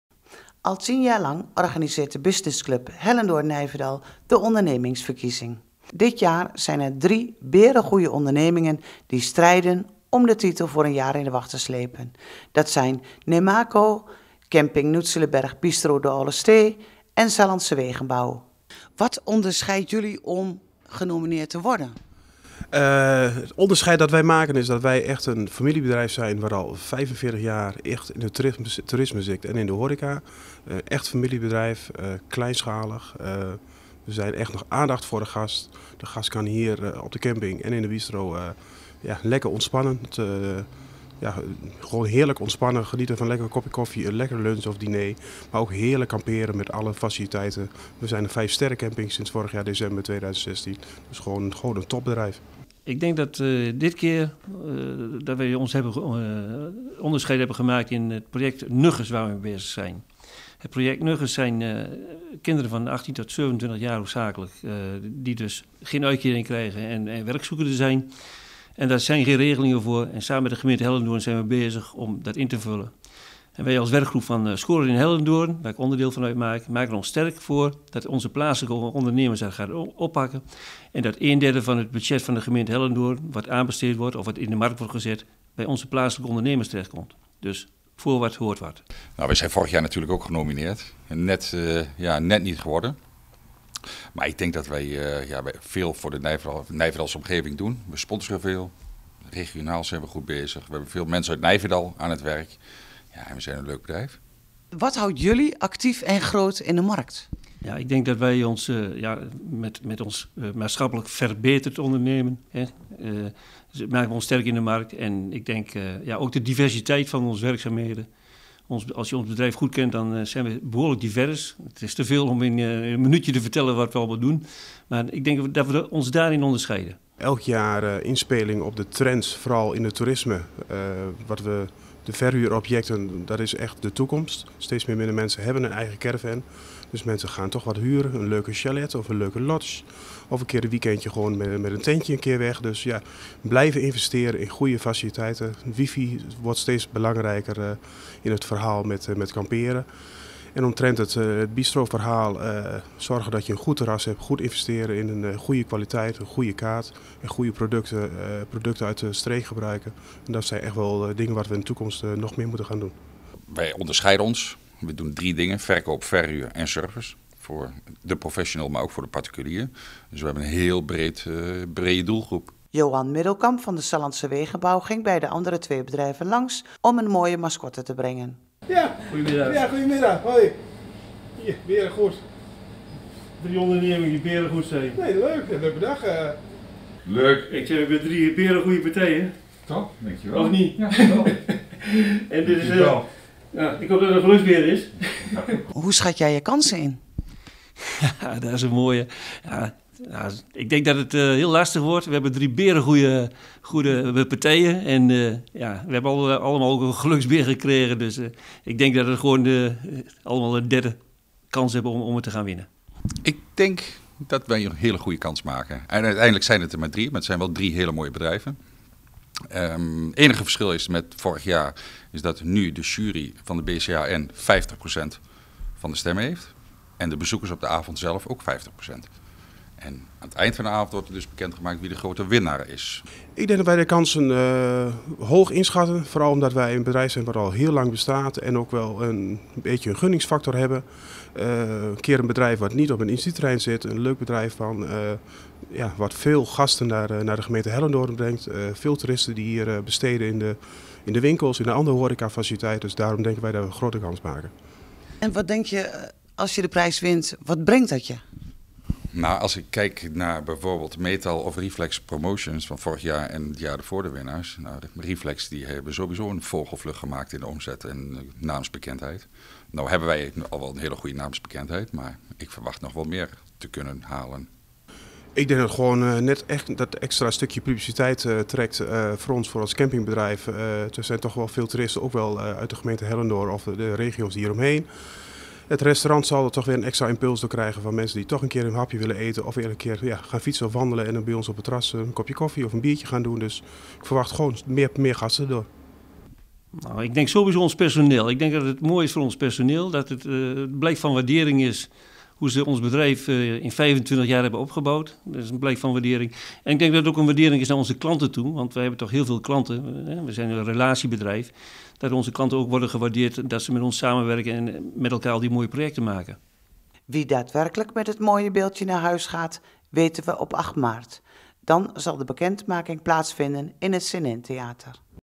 Ondernemingsverkiezing 2017 BCHN
3 geweldige bedrijven uit de gemeente Hellendoorn strijden om de titel Onderneming van het jaar van de BCHN. Dit zijn Sallandse Wegenbouw, Nemaco en camping Noetselerberg / Bistro De Oale Ste. De prijswinnaar zal op 8 maart 2017 worden bekend gemaakt.